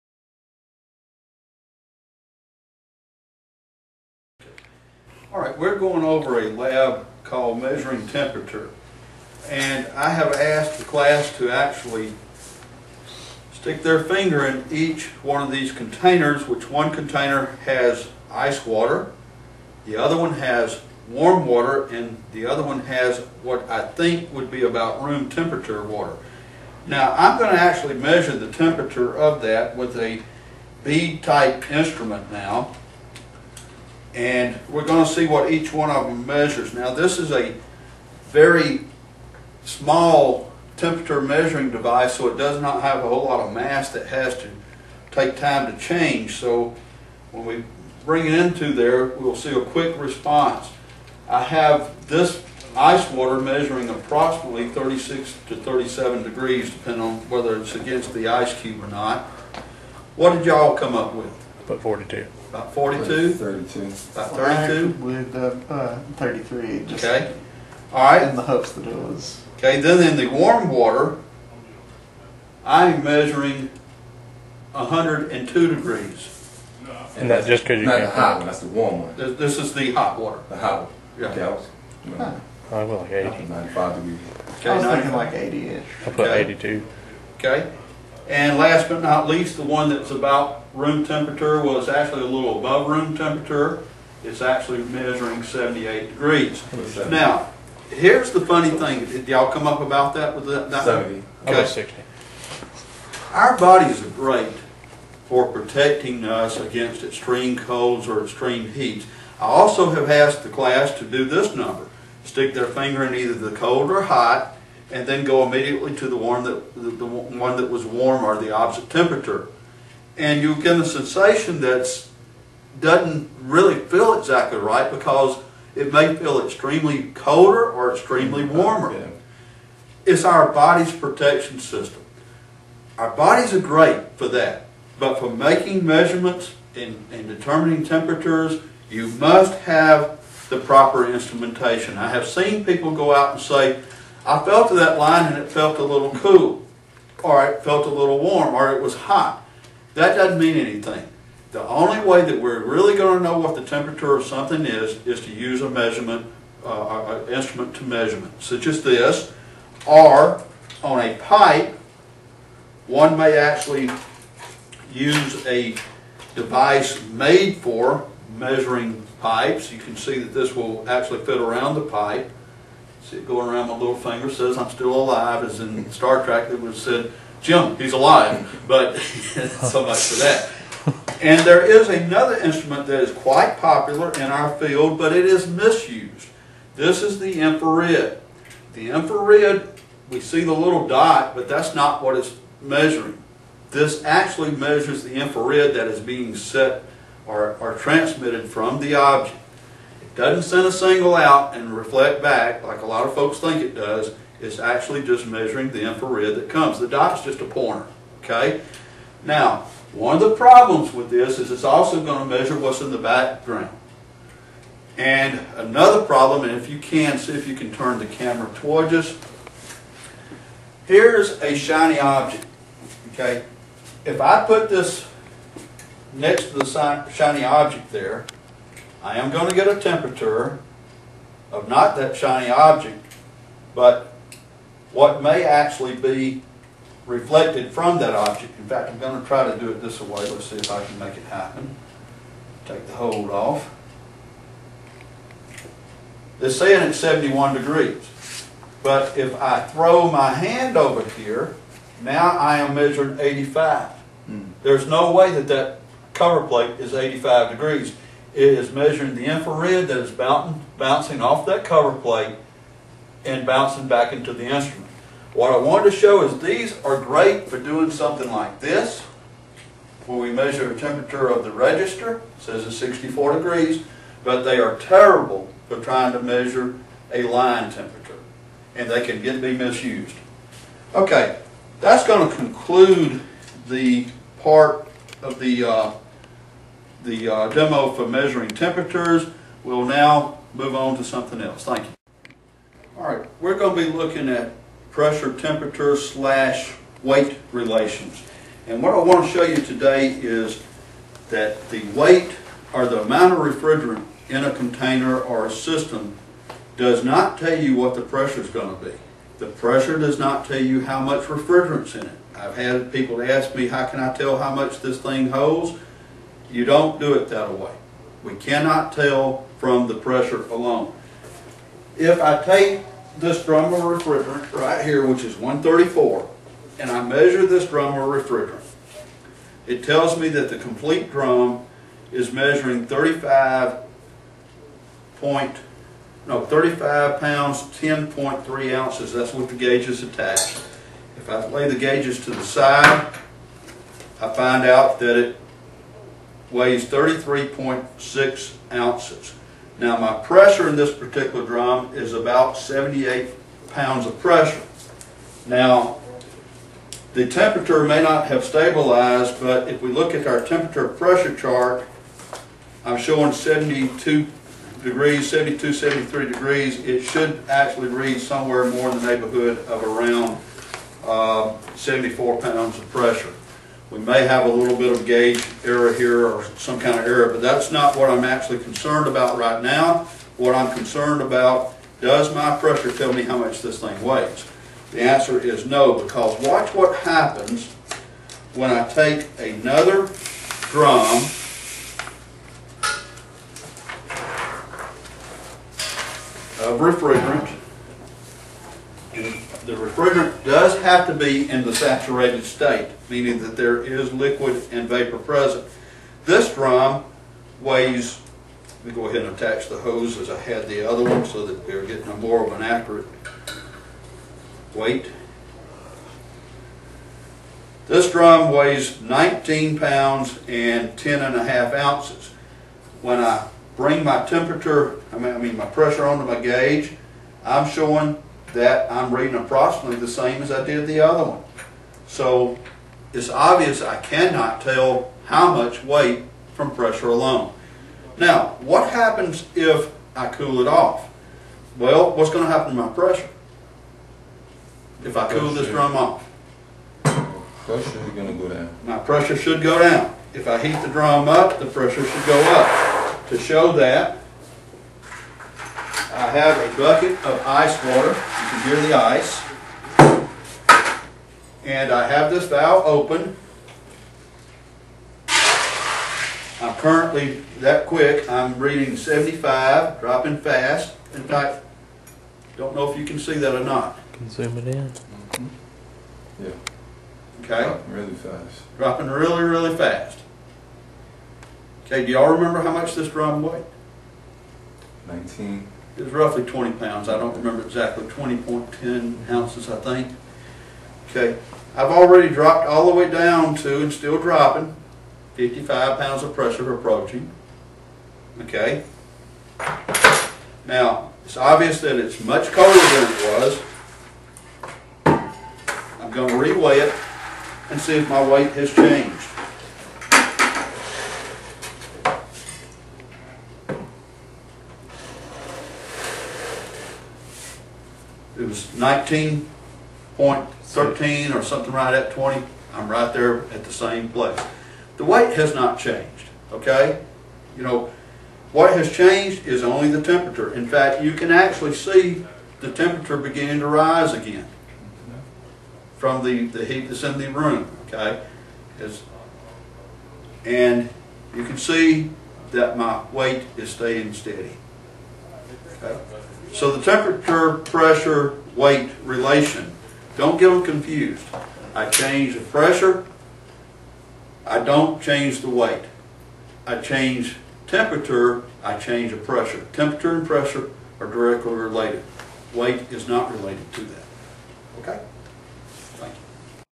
AIRC 1005 B, Part 1 of 1; Temperature, Pressure & Weight Relationships
Instructor David Cooley discusses various methods of measuring temperature, and weight/volume pretaining to the HVACR field.